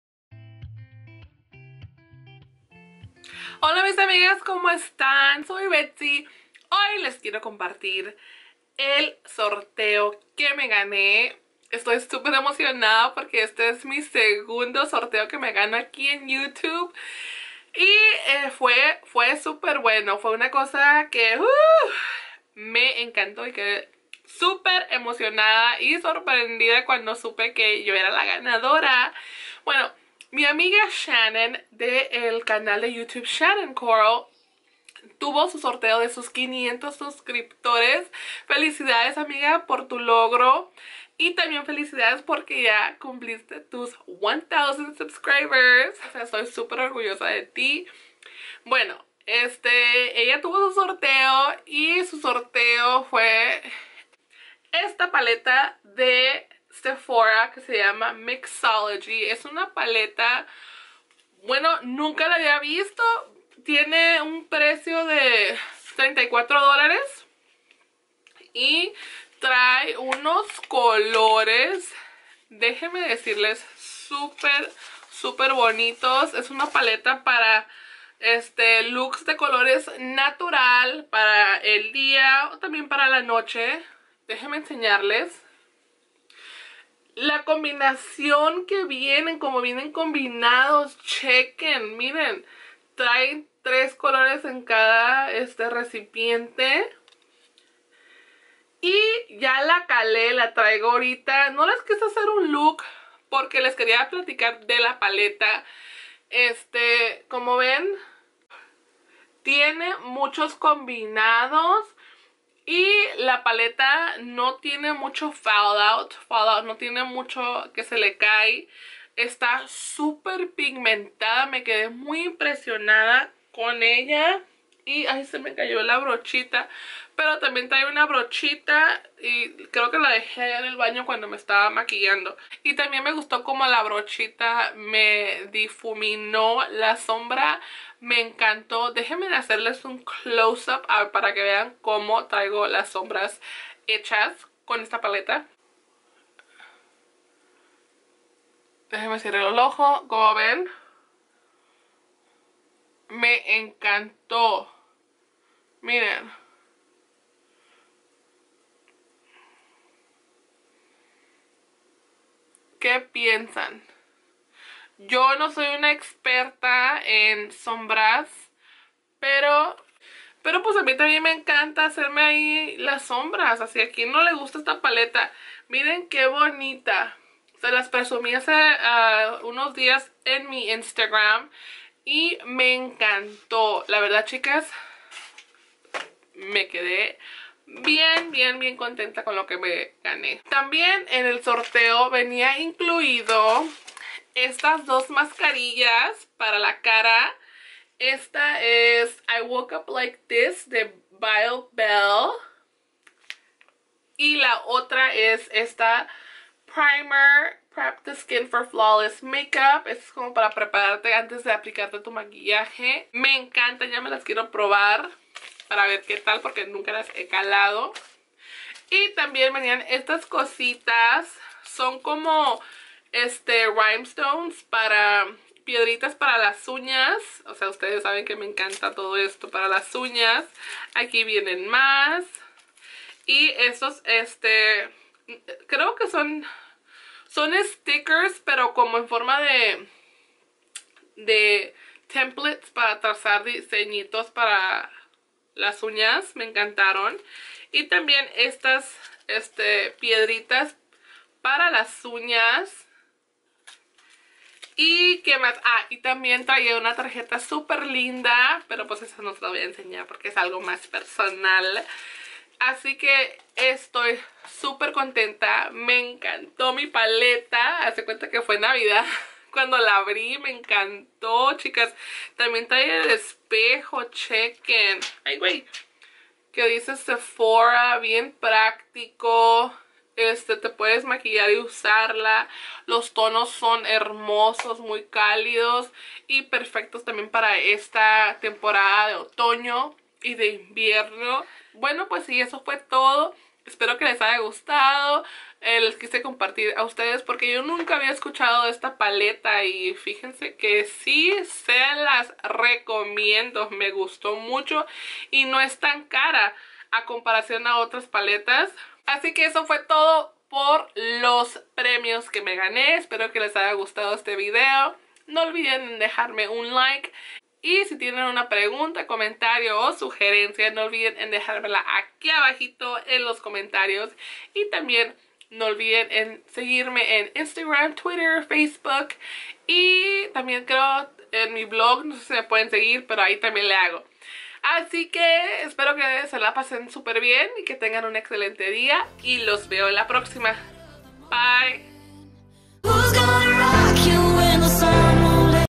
SEPHORA MIXOLOGY PALETA RESEÑA
ME GANE UN SORTEO AQUI EN YOUTUBE Y EN LOS PREMIOS VENIA ESTA HERMOSA PALETA DE SOMBRAS DE SEPHORA MIXOLOGY ME SUPER ENCANTO. ESPERO Y LES GUSTE LA RESEÑA.

SEPHORA MIXOLOGY

Love
Betsy XX